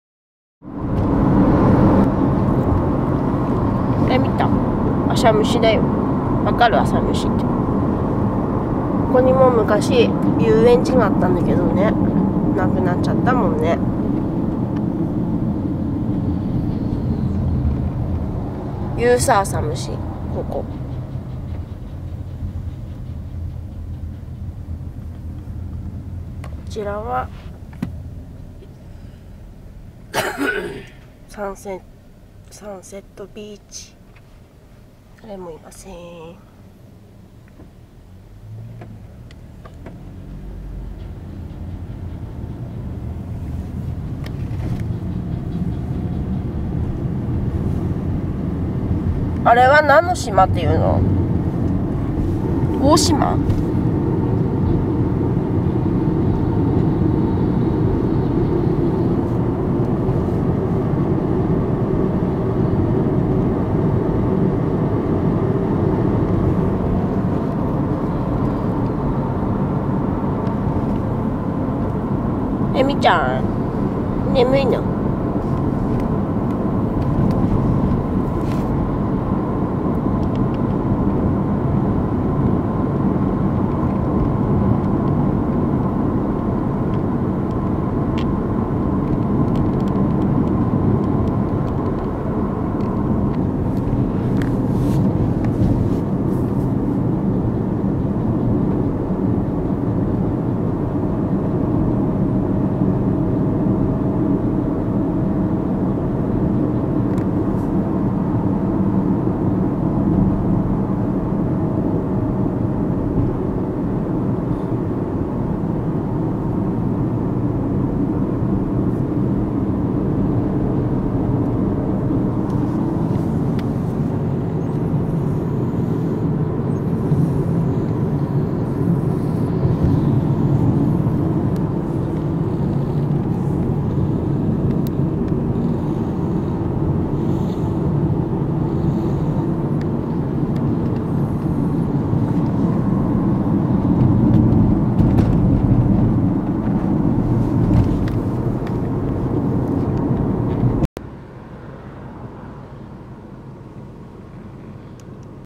旅の途中で同時に眠る三姉妹
2012/4/12
We headed to Hachinohe City, from Aomori City by car.
On the way, three sisters got tired of sleeping at the same time.